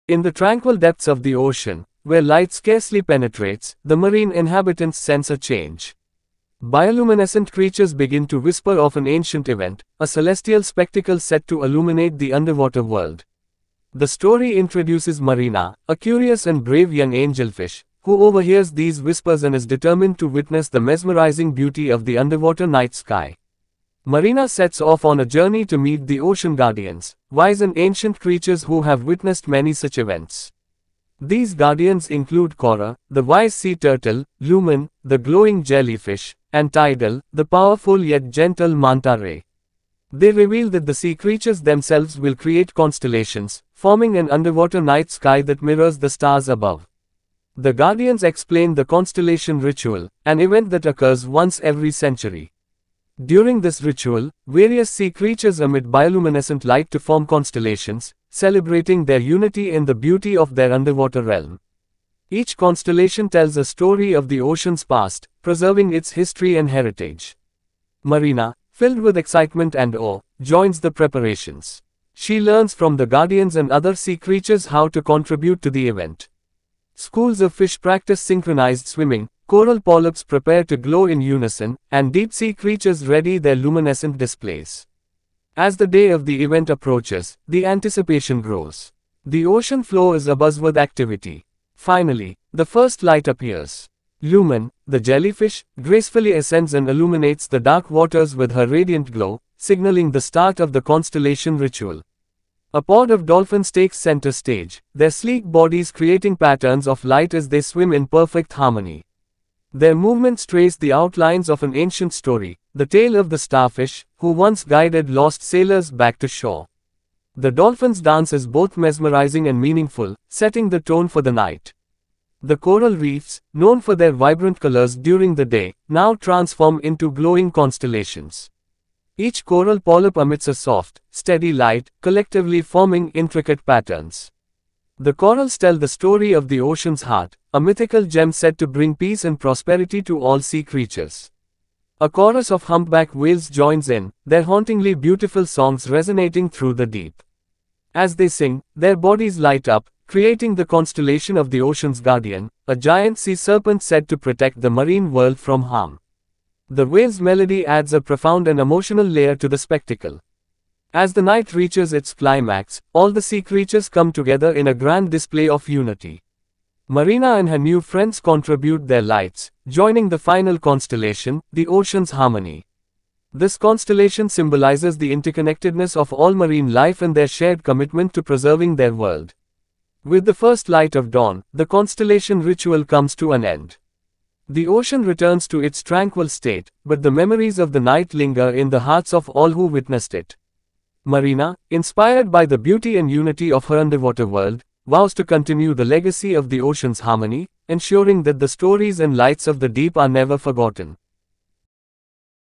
The Grand Finale: Ocean's Harmony |Enjoysongskids kids stories
kidsstoryIn the tranquil depths of the ocean, where light scarcely penetrates, the marine inhabitants sense a change. Bioluminescent creatures begin to whisper of an ancient event, a celestial spectacle set to illuminate the underwater world. The story introduces Marina, a curious and brave young angelfish, who overhears these whispers and is determined to witness the mesmerizing beauty of the underwater night sky.

Marina sets off on a journey to meet the Ocean Guardians, wise and ancient creatures who have witnessed many such events. These guardians include Kora, the wise sea turtle, Lumen, the glowing jellyfish, and Tidal, the powerful yet gentle manta ray. They reveal that the sea creatures themselves will create constellations, forming an underwater night sky that mirrors the stars above.